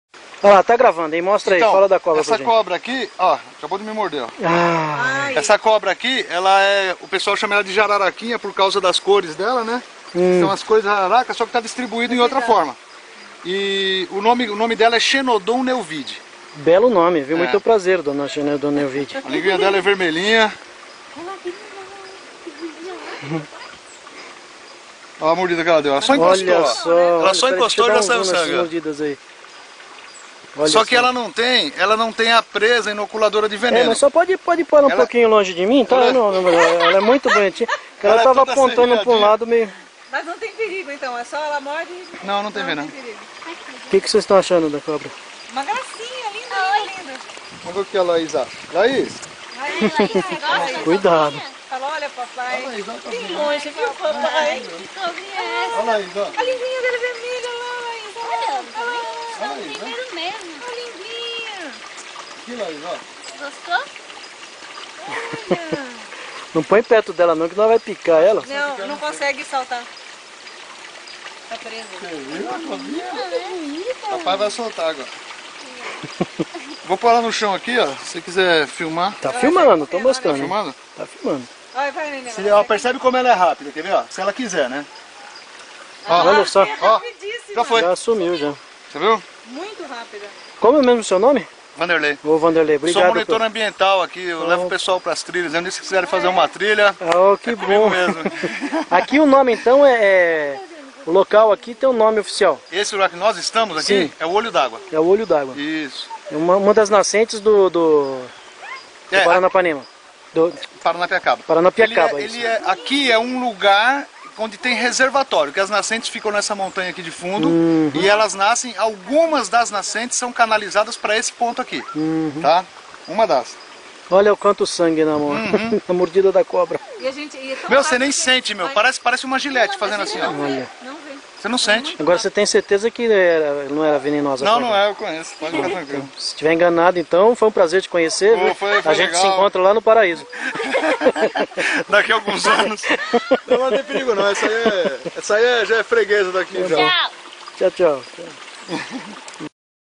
Picada de cobra em Paranapiacaba - Xenodon neuwiedii
Essa foi legal (na realidade não foi uma picada, mas sim, uma mordida) ... eu havia capturado essa serpente que estava no quintal do meu vizinho e quando eu estava indo reintegrá-la à Natureza, encontrei o Camilo com sua família (foi um enorme prazer conhece-los) então o Camilo filmou este evento pitoresco...kkkkkk.
Só um detalhe, se a serpente fosse peçonhenta eu jamais deixaria ela escolher entre morder ou não.
Se fosse peçonhenta eu a transportaria imobilizada, claro.